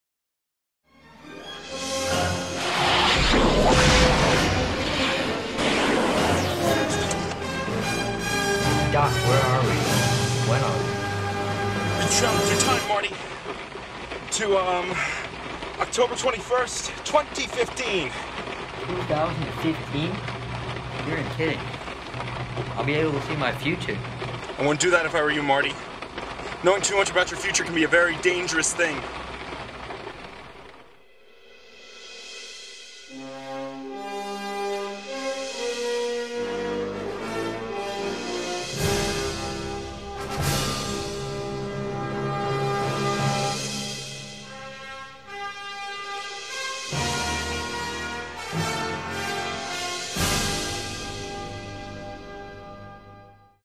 Back to the Future | Video Class Project
Just a little green screen test for school.